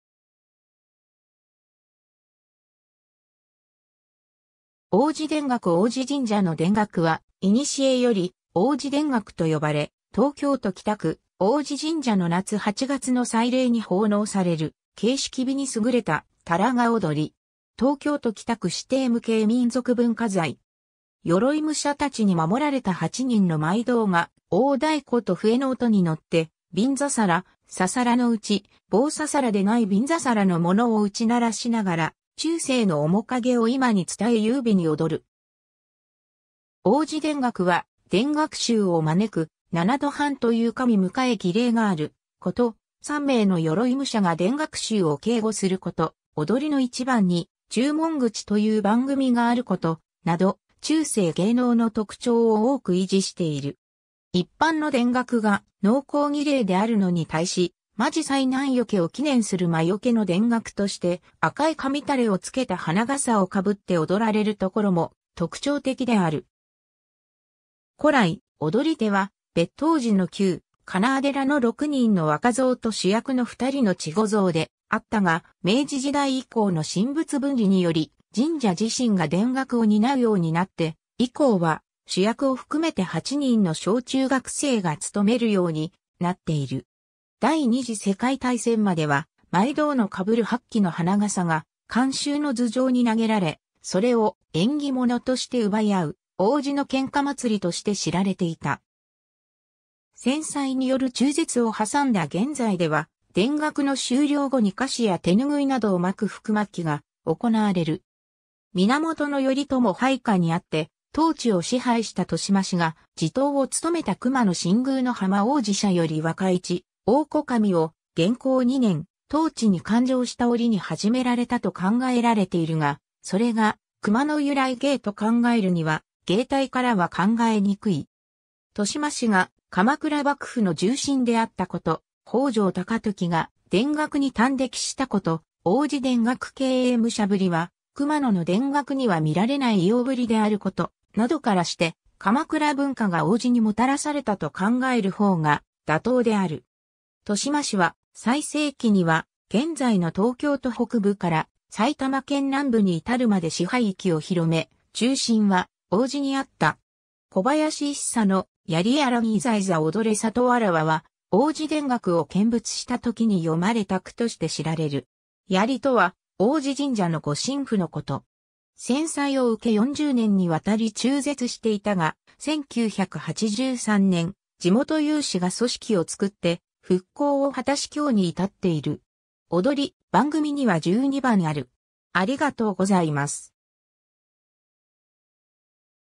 王子神社の田楽
王子田楽 王子神社の田楽（でんがく）は古より王子田楽と呼ばれ、東京都北区・王子神社の夏8月の祭礼に奉納される形式美に優れた田楽踊り。
東京都北区指定無形民俗文化財（民俗芸能、1987年〈昭和62年〉4月1日指定）。
 鎧武者たちに護られた8人の舞童が大太鼓と笛の音にのって、びんざさら(ささらのうち、棒ささらでない編木のもの）を打ち鳴らしながら中世の面影を今に伝え優美に踊る。
王子田楽は、田楽衆を招く「七度半（しちどはん）」という神迎え儀礼があること、三名の鎧武者が田楽衆を警護すること、踊りの一番に「中門口（ちゅうもんぐち）」という番組があること、など中世芸能の特徴を多く維持している。
一般の田楽が農耕儀礼であるのに対し、魔事災難除けを祈念する魔除けの田楽として赤い紙垂れを付けた花笠をかぶって踊られるところも特徴的である。
 古来、踊り手は、別当寺の旧・金輪寺の6人の若僧と主役の2人の稚児僧であったが、明治時代以降の神仏分離により神社自身が田楽を担うようになって以降は、主役を含めて8人の小中学生がつとめるようになっている。
 第二次世界大戦までは舞童のかぶる8基の花笠が観衆の頭上に投げられ、それを縁起物として奪い合う「王子の喧嘩(けんか)祭り」として知られていた。
戦災による中絶を挟んだ現在では、田楽の終了後に菓子や手拭いなどを撒く福まきが行われる。
 源頼朝配下にあって当地を支配した豊島氏が、地頭をつとめた熊野新宮の浜王子社より若一王子（にゃくいち）神を元亨2年（1322年）、当地に勧請した折に始められたと考えられているが、それが熊野由来芸と考えるには芸態からは考えにくい。
豊島氏が鎌倉幕府の重臣であったこと、北条高時が田楽に耽溺したこと、王子田楽警衛武者ぶりは熊野の田楽には観られない偉容ぶりであること、などからして鎌倉文化が王子にもたらされたと考えるほうが妥当である。
豊島氏は、最盛期には現在の東京都北部から埼玉県南部に至るまで支配域を広め、中心は王子にあった。
 小林一茶の「 鑓（やり）やらん　いざいざおどれ　里わらわ」は王子田楽を見物したときに詠まれた句として知られる。
「鑓」とは王子神社の御神符のこと。
 戦災を受け40年にわたり中絶していたが、1983年（昭和58年）、地元有志が組織を作って復興を果たし今日に至っている。
 躍り番組には十二番ある。